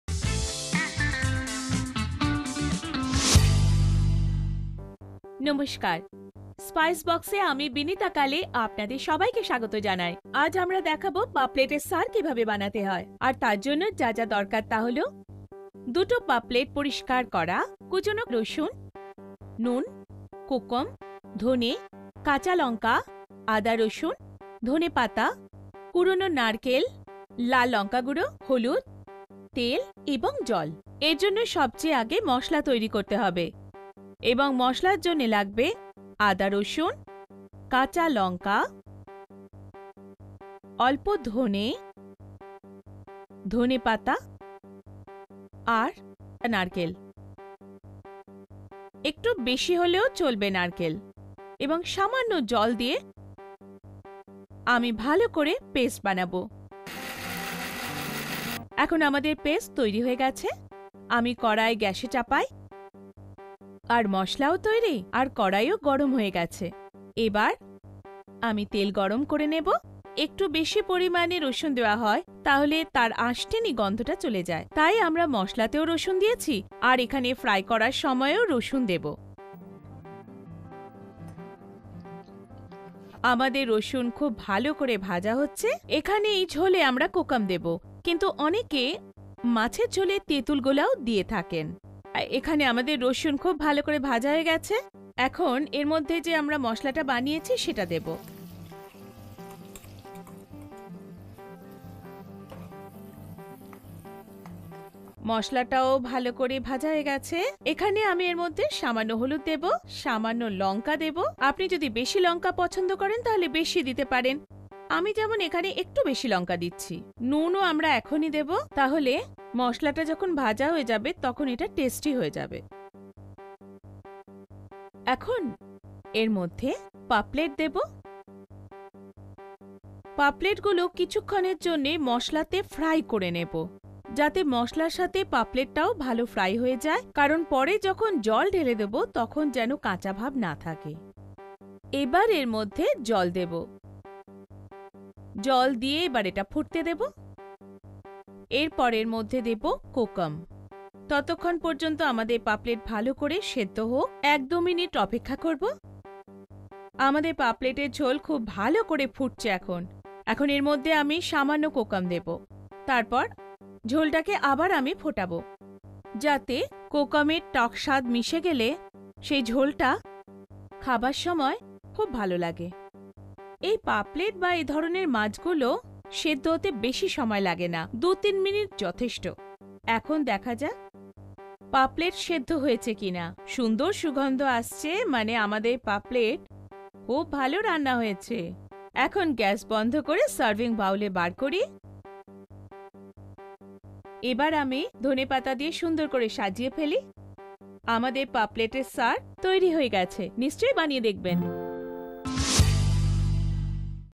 পমফ্রেট কারি (Paplet saar)
উপকরণ -

পমফ্রেট ২টো টুকরো করে কাটা
রসুন কুচি ১ চামচ
কোকাম ৫-৫টি
গোটা ধনে ১ চামচ
কাঁচালঙ্কা ৭-৮টি
আদার টুকরো ৩-৪টি
রসুনের কোয়া ৮-১০টি
ধনেপাতা কুচি ২ চামচ
নারকেল কোরানো ১ কাপ
শুকনো লঙ্কা গুঁড়ো ১ চামচ
হলুদ গুঁড়ো ১ চামচ
নুন স্বাদমতো
তেল পরিমাণমতো
জল প্রয়োজনমতো

পদ্ধতি -

মিক্সারে আদা, রসুন, কাঁচালঙ্কা, গোটা ধনে, ধনেপাতা, কোরানো নারকেল নিন।
সামান্য জল দিয়ে মশলা বেটে নিন।
গ্যাসে প্যান বসিয়ে তেল গরম করুন।
তেল গরম হলে প্যানে কোচানো রসুন দিয়ে ভেজে নিন।
রসুন ভাজা হলে বেটে রাখা মশলা দিয়ে ভাজতে থাকুন।
মশলা ভাজা হলে প্যানে দিন হলুদ গুঁড়ো, শুকনো লঙ্কা গুঁড়ো, নুন।
মশলা কষে গেলে প্যানে পমফ্রেটের টুকরো দিয়ে ভেজে নিন।
এরপর প্যানে জল দিয়ে ঢেকে রাখুন।
পমফ্রেট সেদ্ধ হলে কয়েকটি কোকাম দিয়ে গ্রেভি ফোটাতে থাকুন।
মাছ সেদ্ধ হয়ে গেলে গ্যাস বন্ধ করে দিলেই তৈরি পমফ্রেট কারি।
কোচানো ধনেপাতা দিয়ে গর্নিশ করুন।